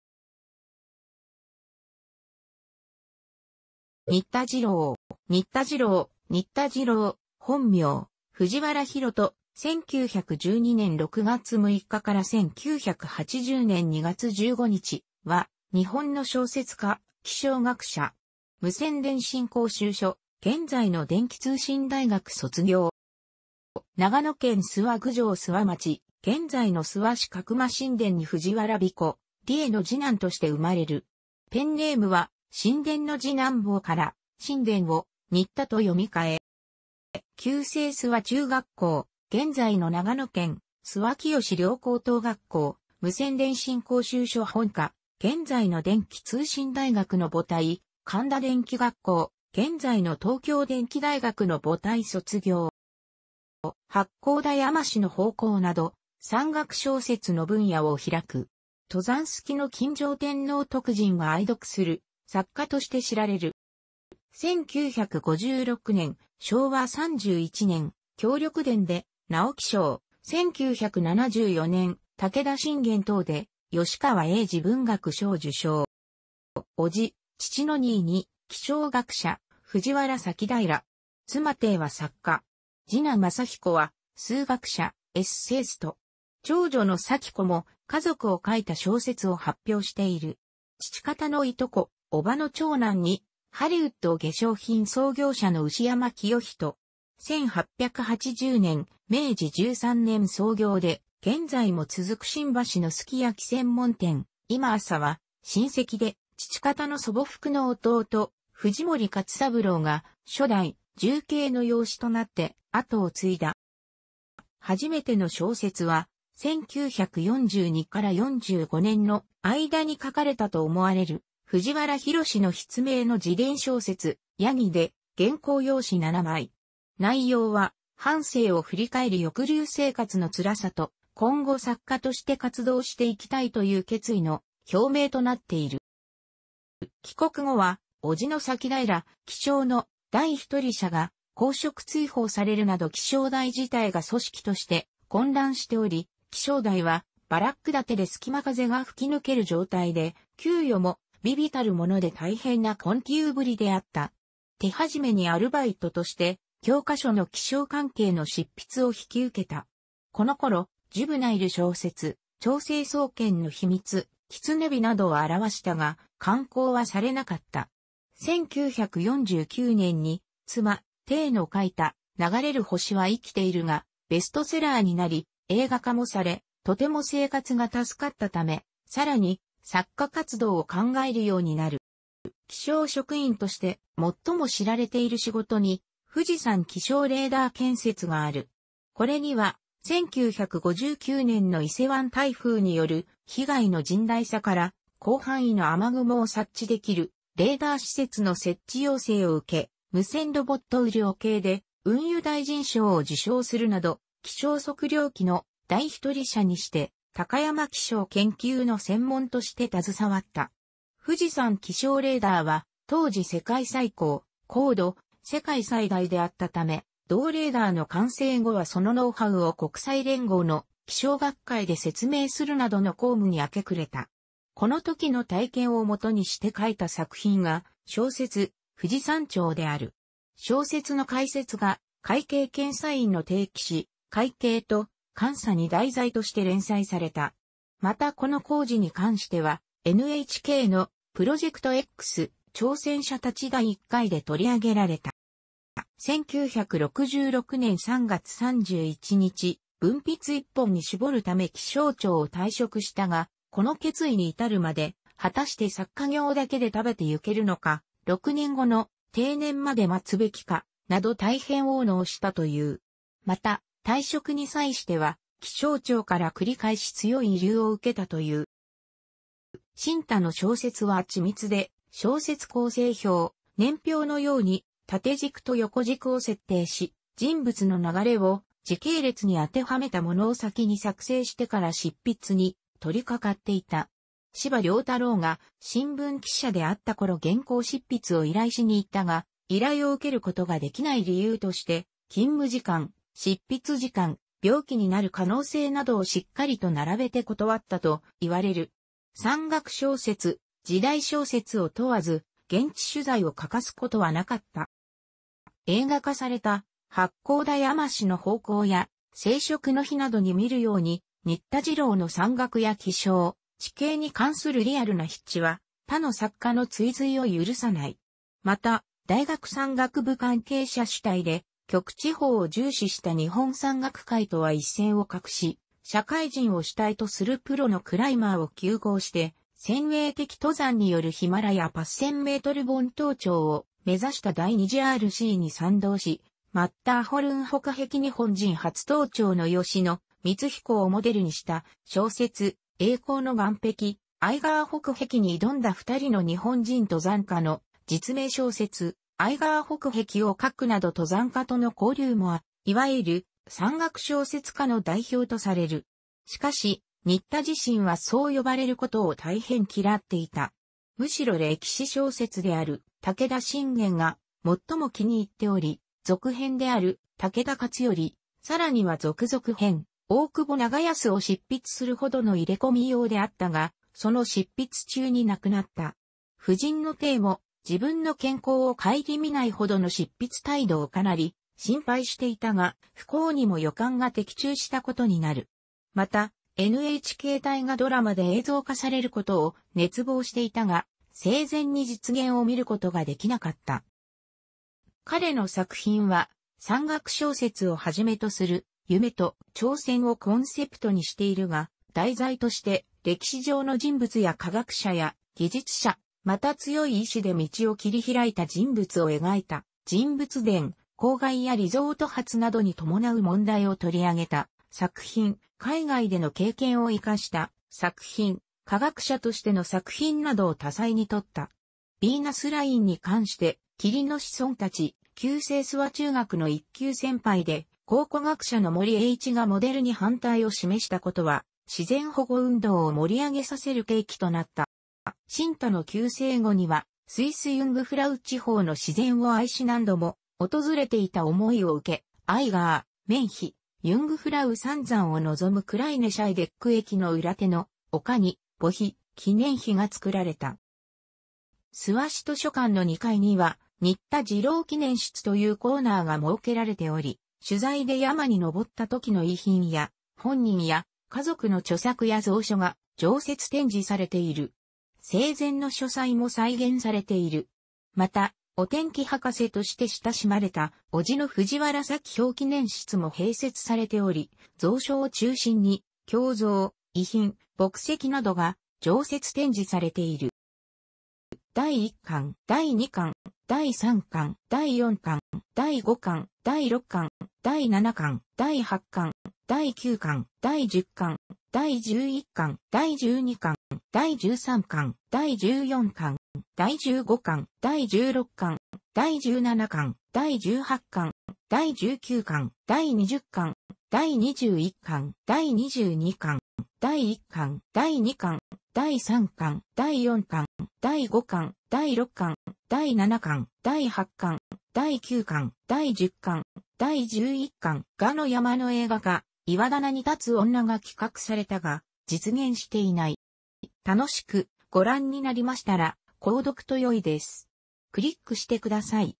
新田次郎
新田 次郎（にった じろう、本名：藤原 寛人（ふじわら ひろと）、1912年6月6日 - 1980年2月15日）は、日本の小説家、気象学者。無線電信講習所（現在の電気通信大学）卒業。

長野県諏訪郡上諏訪町（現在の諏訪市）角間新田（かくましんでん）に藤原彦、りゑの次男として生まれる。ペンネームは“新田の次男坊”から（「しんでん」を「にった」と読み替え）。
旧制諏訪中学校（現在の長野県諏訪清陵高等学校）・無線電信講習所本科（現在の電気通信大学の母体）・神田電機学校（現在の東京電機大学の母体）卒業。

『八甲田山死の彷徨』など山岳小説の分野を拓く。登山好きの今上天皇徳仁が愛読する作家として知られる。

1956年（昭和31年）『強力伝』で直木賞、1974年『武田信玄』等で吉川英治文学賞受賞。

伯父（父の兄）に気象学者藤原咲平。妻ていは作家。次男正彦は数学者・エッセイスト。長女の咲子も、家族を書いた小説を発表している。父方のいとこ（叔母の長男）に、ハリウッド化粧品創業者の牛山清人。1880年（明治13年）創業で現在も続く新橋のすき焼き専門店「今朝（いまあさ）」は親戚で、父方の祖母ふくの弟・藤森勝三郎が初代（従兄）の養子となって跡を継いだ。

初めての小説は、1942～45年の間に書かれたと思われる、藤原廣の筆名の自伝小説『山羊』で原稿用紙7枚。内容は、半生を振り返り抑留生活の辛さと、今後作家として活動していきたいという決意の表明となっている。

帰国後は、伯父の咲平（気象の第一人者）が公職追放されるなど気象台自体が組織として混乱しており、気象台はバラック立てで隙間風が吹き抜ける状態で、給与も微々たる物で大変な困窮ぶりであった。手始めにアルバイトとして、教科書の気象関係の執筆を引き受けた。このころ、ジュブナイル小説『超成層圏の秘密』『狐火』などを著したが、刊行はされなかった。1949年に、妻・ていの書いた『流れる星は生きている』がベストセラーになり映画化もされ、とても生活が助かったため、さらに、作家活動を考えるようになる。

気象職員として最も知られている仕事に、富士山気象レーダー建設がある。これには、1959年の伊勢湾台風による被害の甚大さから、広範囲の雨雲を察知できるレーダー施設の設置要請を受け、無線ロボット雨量計で運輸大臣賞を受賞するなど、気象測量機の第一人者にして高山気象研究の専門として携わった。富士山気象レーダーは当時世界最高（高度）・世界最大であったため、同レーダーの完成後はそのノウハウを国際連合の気象学会で説明するなどの公務に明け暮れた。この時の体験を基にして書いた作品が、小説『富士山頂』である。小説の解説が、会計検査院の定期誌「会計と監査」に題材として連載された。またこの工事に関しては、NHKの『プロジェクトX〜挑戦者たち〜』第1回で取り上げられた。

1966年3月31日、文筆一本に絞るため気象庁を退職したが、この決意に至るまで、果たして作家業だけで食べてゆけるのか、6年後の定年まで待つべきか、など大変懊悩したという。また、退職に際しては気象庁から繰り返し強い慰留を受けたという。

新田の小説は緻密で、小説構成表（年表のように縦軸と横軸を設定し、人物の流れを時系列に当てはめたもの）を先に作成してから執筆に取り掛かっていた。司馬遼太郎が新聞記者であった頃原稿執筆を依頼しに行ったが、依頼を受けることができない理由として勤務時間・執筆時間・病気になる可能性などをしっかりと並べて断ったと言われる。山岳小説、時代小説を問わず、現地取材を欠かすことはなかった。

映画化された『八甲田山死の彷徨』や『聖職の碑』などに見るように、新田次郎の山岳や気象、地形に関するリアルな筆致は、他の作家の追随を許さない。また、大学山岳部関係者主体で極地法を重視した日本山岳会とは一線を画し、社会人を主体とするプロのクライマーを糾合して尖鋭的登山によるヒマラヤ8000メートル峰登頂をめざした第2次RCCに賛同し、マッターホルン北壁日本人初登頂の芳野満彦をモデルにした小説『栄光の岩壁』、アイガー北壁に挑んだ2人の日本人登山家の実名小説『アイガー北壁』を書くなど登山家との交流もあり、いわゆる「山岳小説家」の代表とされる。しかし、新田自身はそう呼ばれることを大変嫌っていた。むしろ歴史...